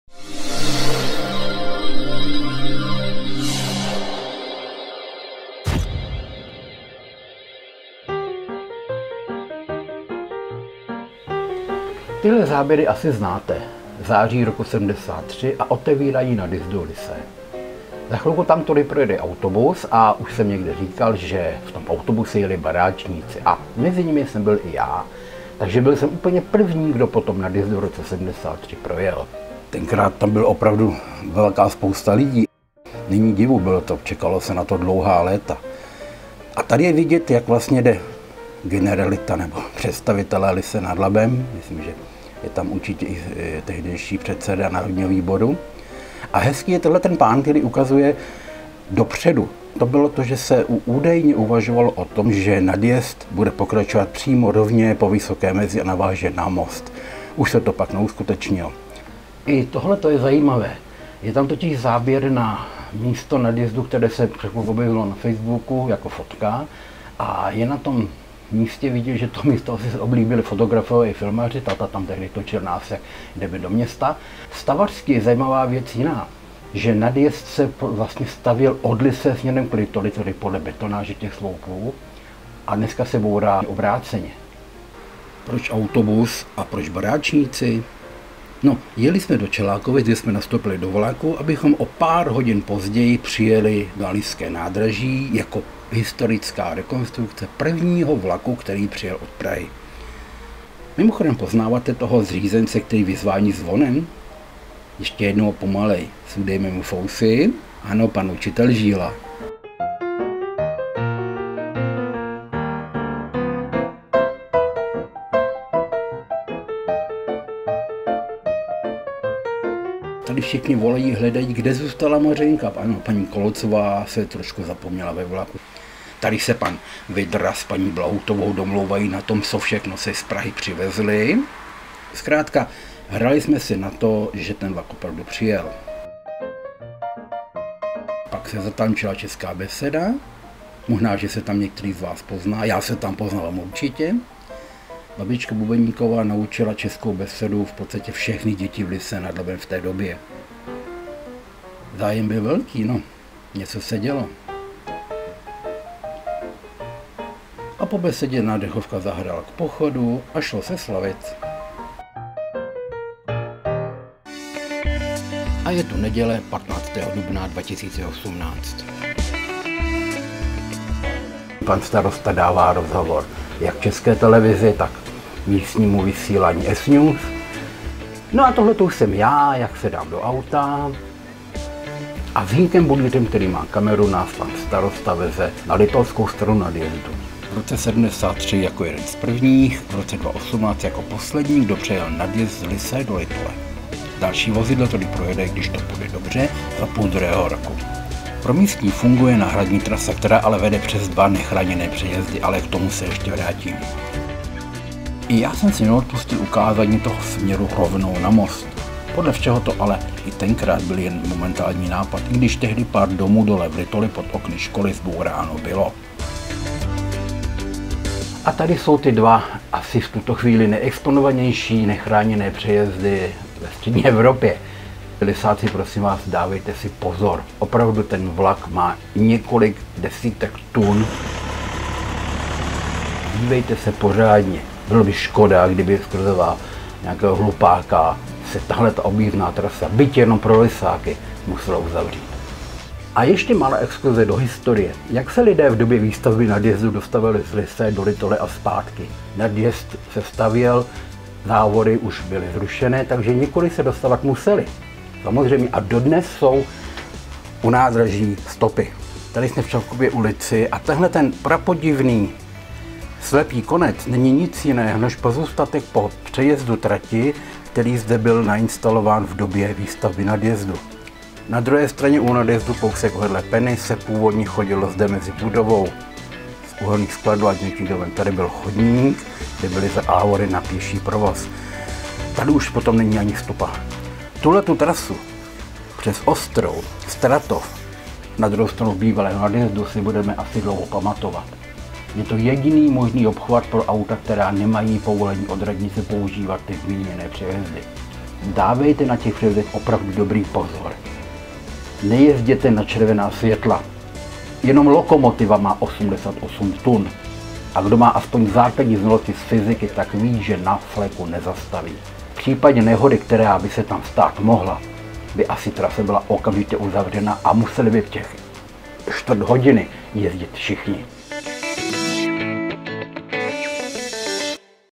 Prvni a posledni
V roce 1973 jsem byl mezi prvími, kdo jel přes nově otevřený nadjezd mezi Lysou nad Labem a Litolí.
V neděli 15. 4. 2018 jsem byl poslední, kdo se po něm dostal přes koleje.